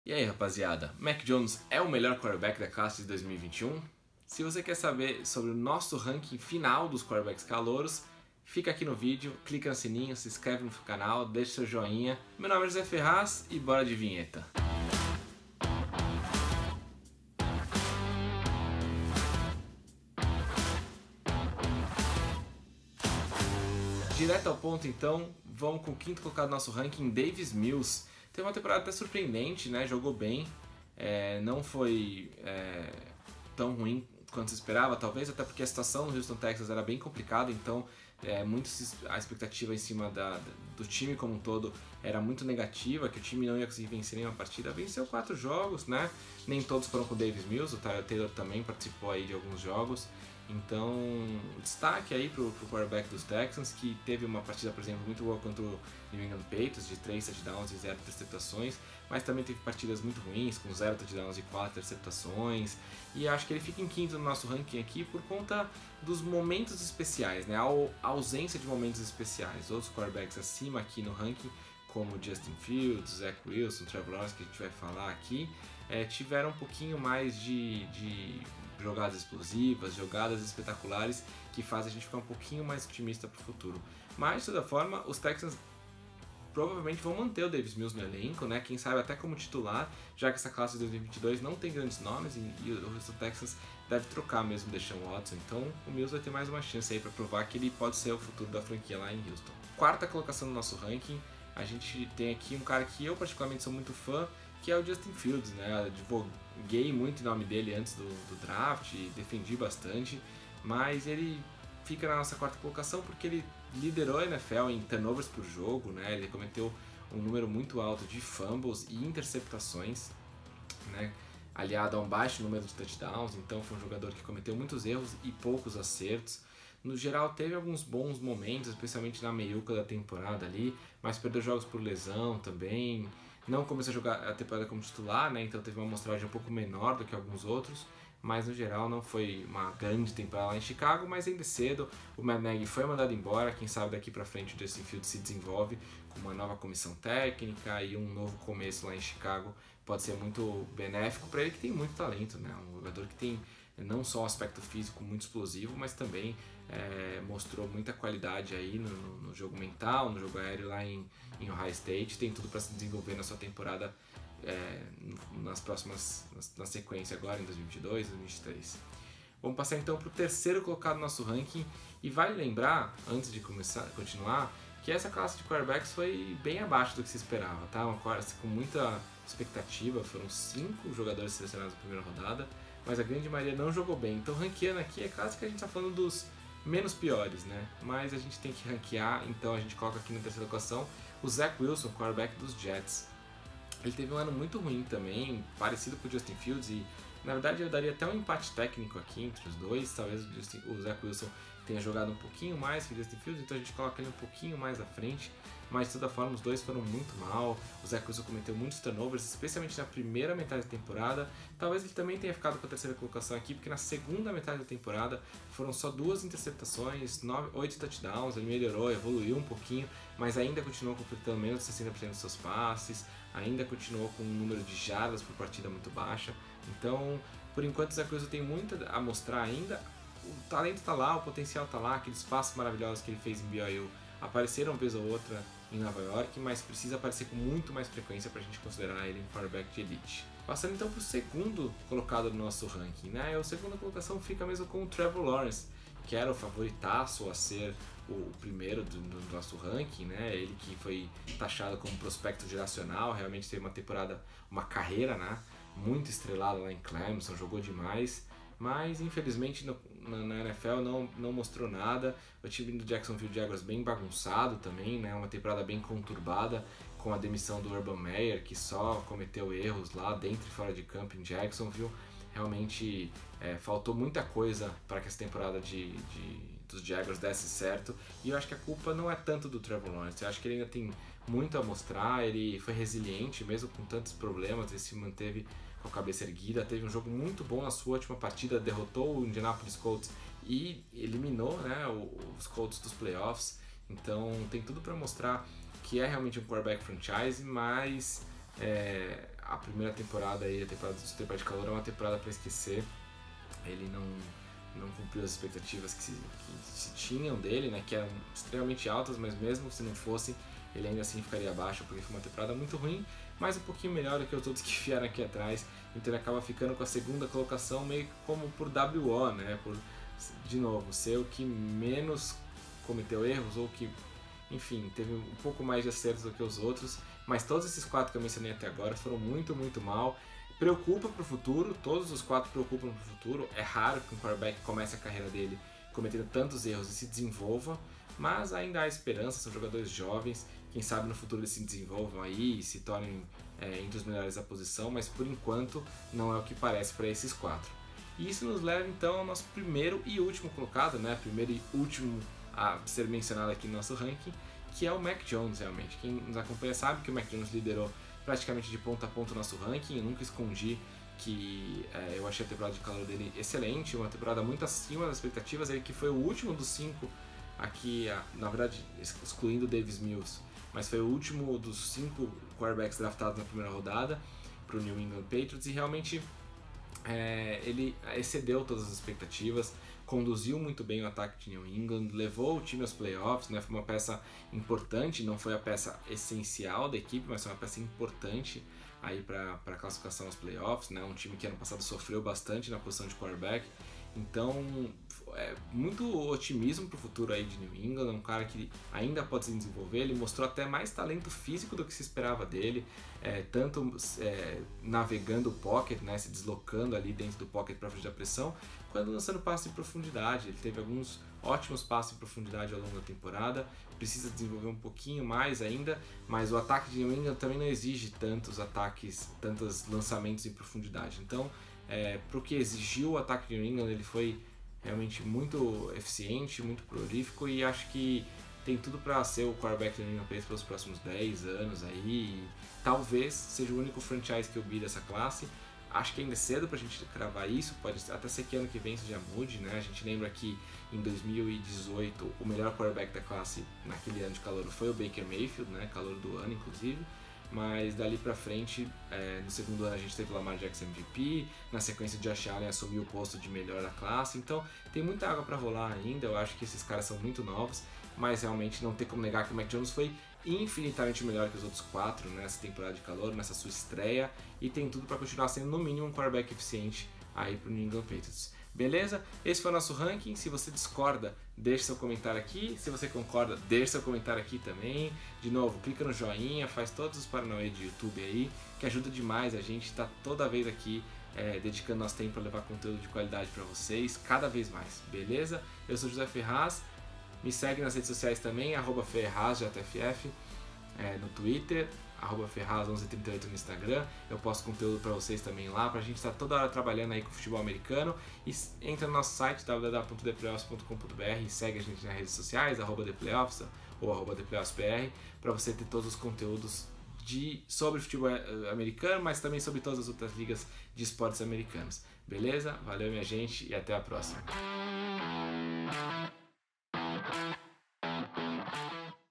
QUEM FOI O MELHOR QUARTERBACK CALOURO DE 2021? (RANKING FINAL)
A temporada está chegando ao fim e é hora de José Ferraz fechar o ranking dos quarterbacks calouros de 2021 da NFL! Mac Jones ou Trevor Lawrence na primeira posição? Confira a lista dos melhores QBs novatos da temporada.

Edição: Lola Produções

Já se inscreveu aqui no canal? Não? ENTÃO SE INSCREVE AGORA!